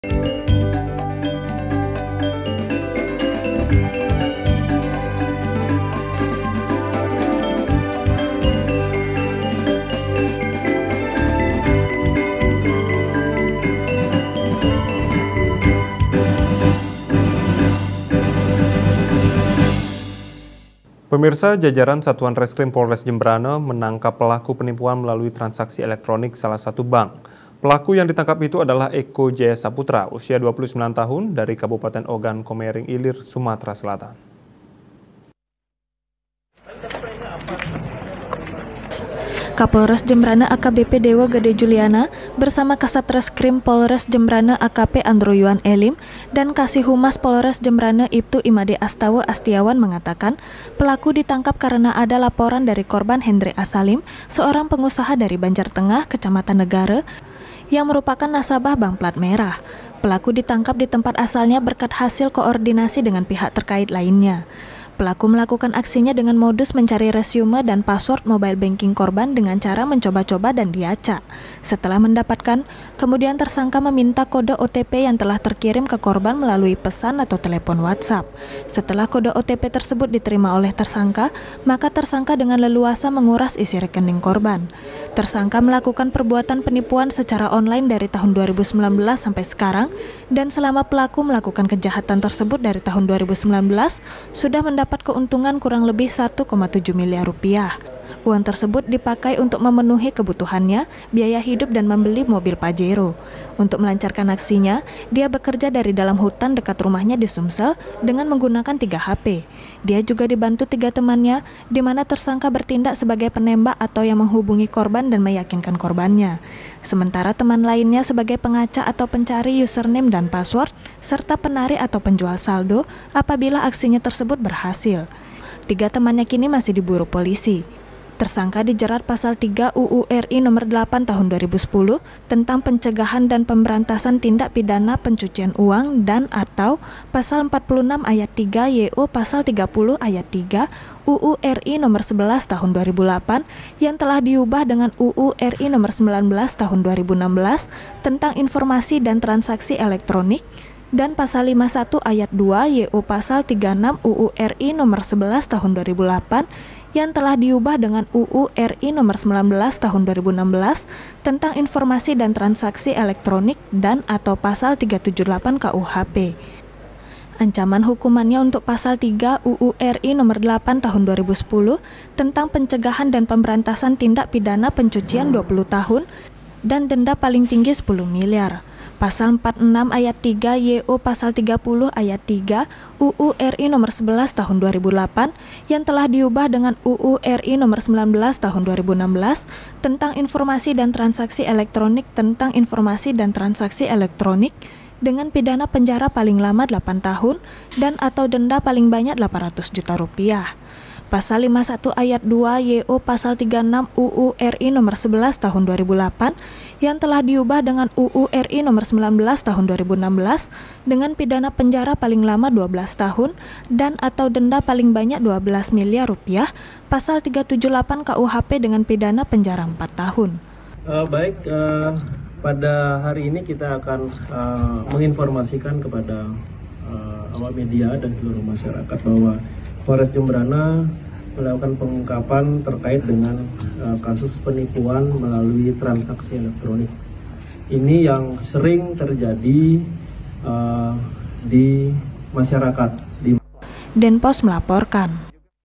PELAKU PENIPUAN MELALUI TRANSAKSI ELEKTRONIK DITANGKAP
JAJARAN SATUAN RESKRIM POLRES JEMBRANA, MENANGKAP PELAKU PENIPUAN MELALUI TRANSAKSI ELEKTRONIK SALAH SATU BANK. PELAKU YANG DITANGKAP ITU, ADALAH EKO JAYA SAPUTRA, USIA 29 TAHUN, DARI KABUPATEN OGAN KOMERING ILIR, SUMATERA SELATAN.